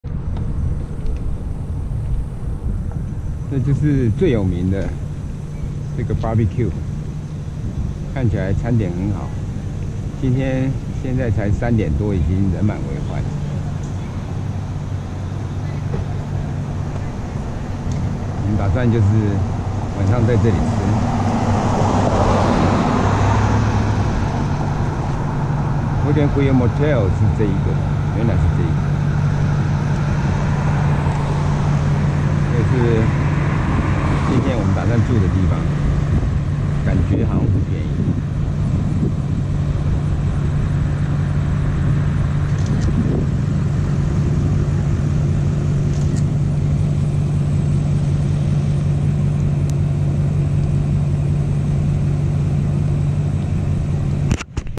RTE-66 20190824 Cuba, MO Wagon Wheel Motel
鄉下地方難得看到停車場停那麼多車 晚餐值得期待